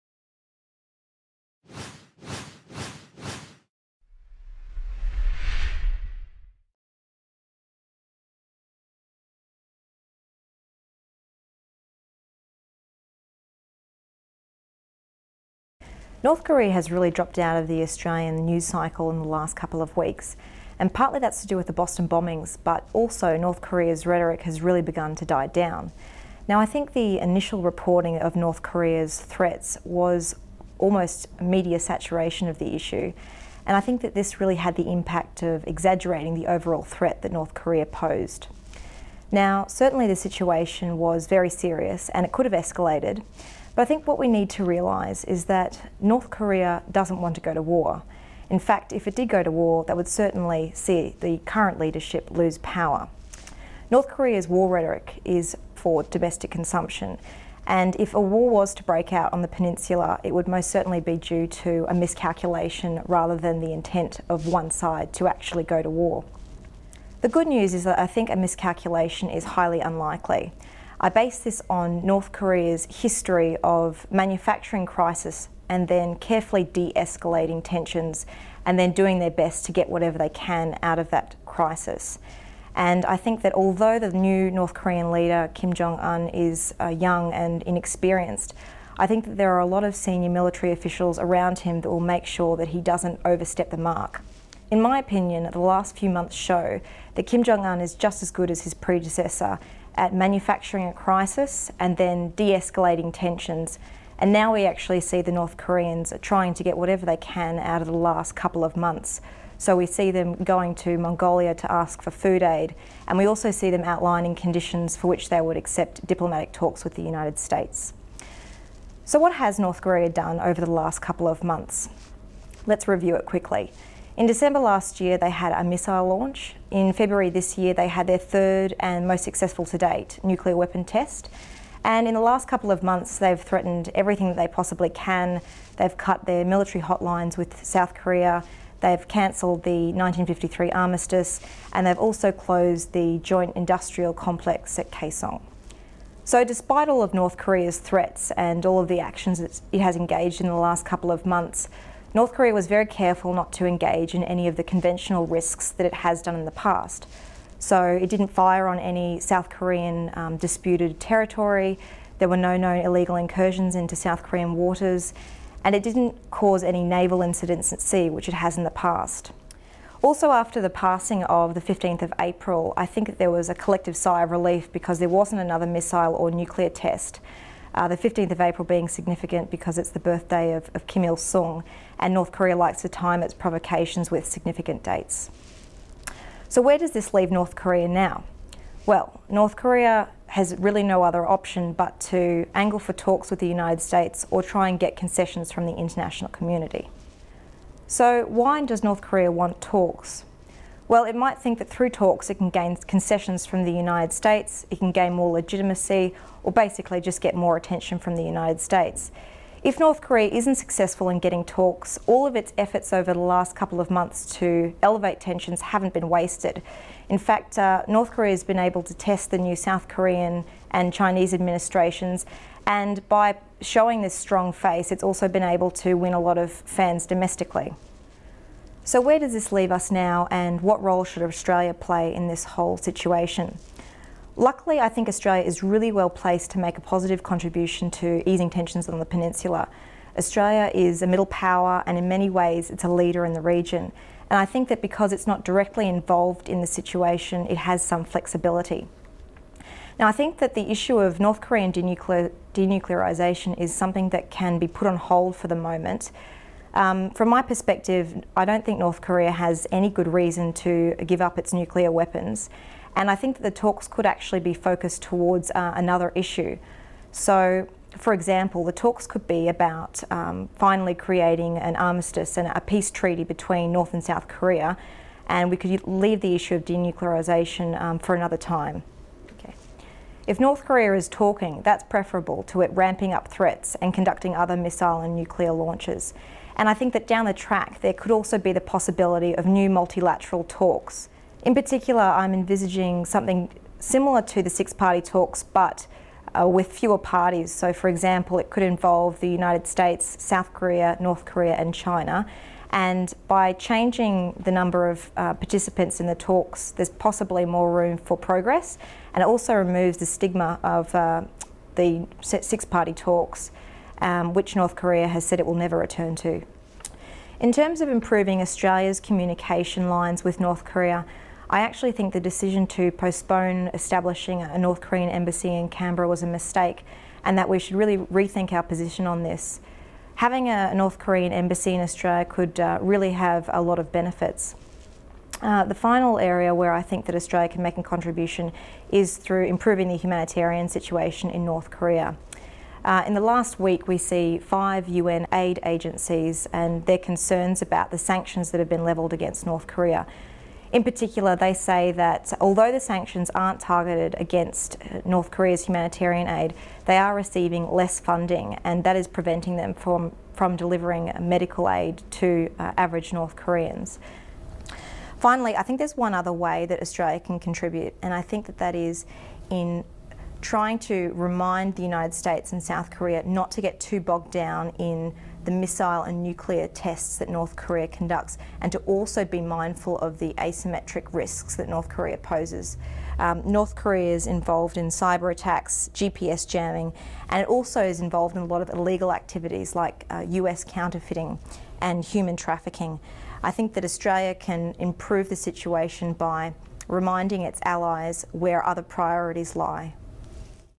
North Korea: Crisis or common-place? Hayley Channer, ASPI
Australian Strategic Policy Institute analyst Hayley Channer gives her thoughts on North Korea's behaviour over recent months.